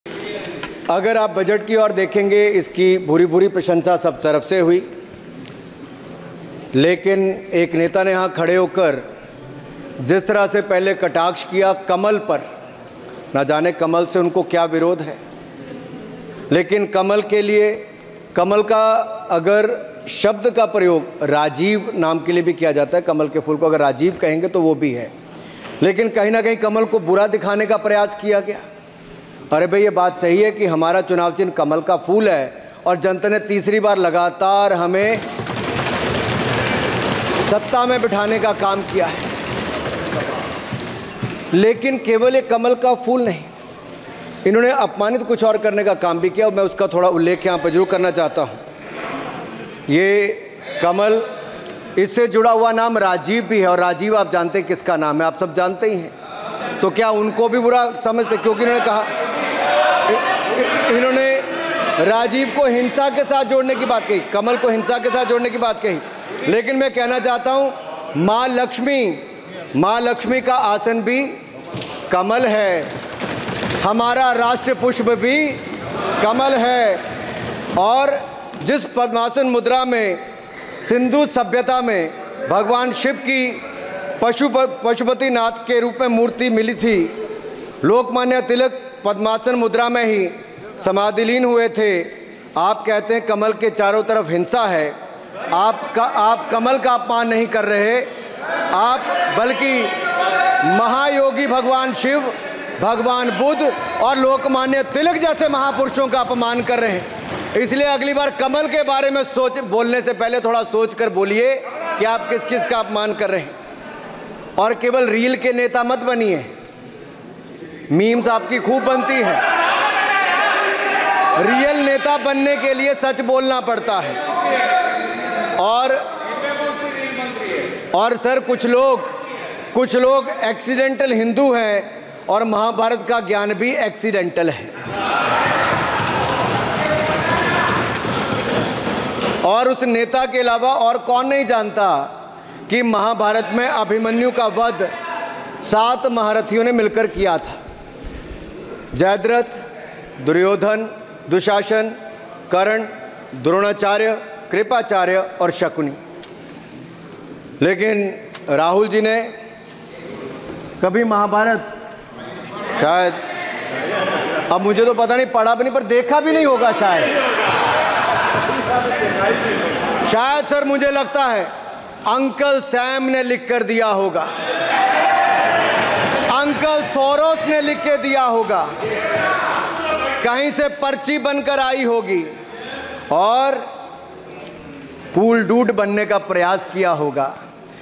Rahul Gandhi ने ऐसा क्या कहा कि Anurag Thakur ने बात उनके पिता Rajiv Gandhi तक पहुंचा दी ?
Anchor Name : | Show Name : | Time : 00: 00 AM/PM | Date : 00/00/0000 | Published By: